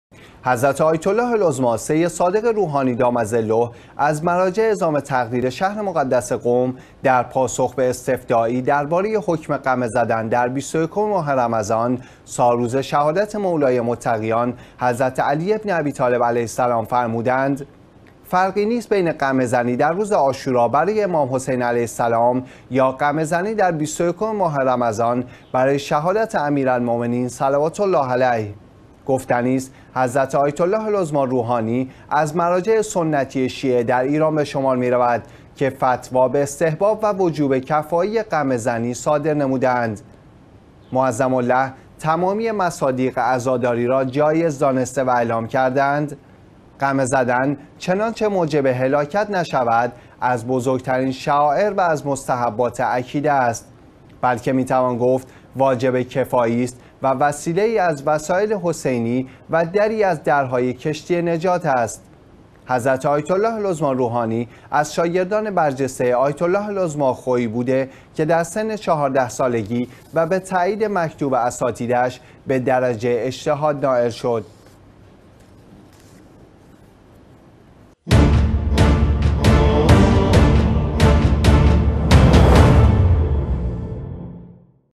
حضرت آیت الله العظمی روحانی: فرقی بین قمه‌زنی در روز عاشورا با قمه زنی در ۲۱ ماه رمضان نیست
حضرت آیت الله العظمی سید صادق روحانی دام ظله، از مراجع عظام تقلید شهر مقدس قم، در پاسخ به استفتائی درباره حکم قمه زدن در ۲۱ ماه رمضان، سالروز شهادت مولای متقیان، حضرت علی بن ابی‌طالب علیه السلام، فرمودند: فرقی نیست بین قمه زنی در روز عاشورا برای امام حسین علیه السلام یا قمه زنی در ۲۱ ماه رمضان برای شهادت امیرالمومنین صلوات الله علیه.
گفتنی است، حضرت آیت الله العظمی روحانی از مراجع سنتی شیعه در ایران به شمار می‌رود که فتوا به استحباب و وجوب کفایی قمه زنی صادر نموده‌اند.
معظم له تمامی مصادیق عزاداری را جایز دانسته و اعلام کرده‌اند: قمه زدن چنانچه موجب هلاکت نشود، از بزرگ‌ترین شعائر و از مستحبات اکیده است، بلکه می‌توان گفت واجب کفایی است و وسیله‌ای از وسائل حسینی و دری از درهای کشتی نجات است.
حضرت آیت الله العظمی روحانی از شاگردان برجسته آیت الله العظمی خویی بوده که در سن 14 سالگی و به تائید مکتوب اساتیدش به درجه اجتهاد نائل شد.